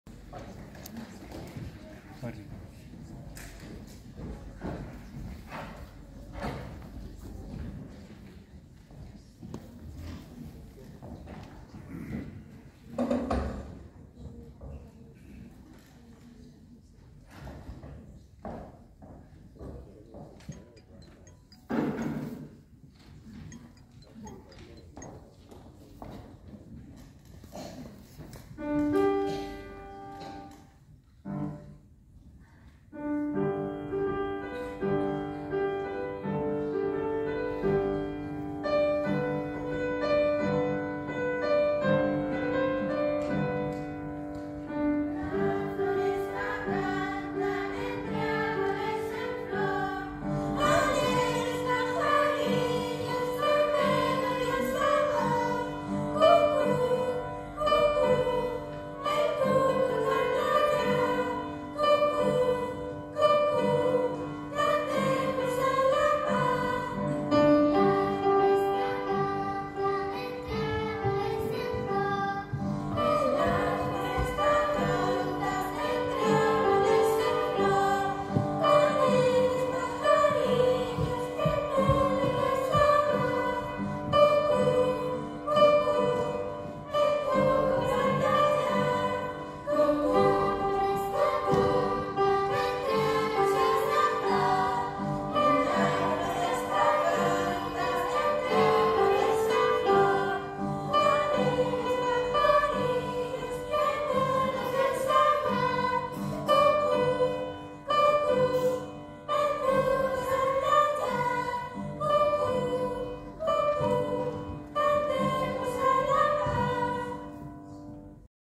221122 - Santa Cecilia - EMMDA
Concierto en honor a Santa Cecilia.
Auditorio Paco de Lucía. 22 de noviembre de 2022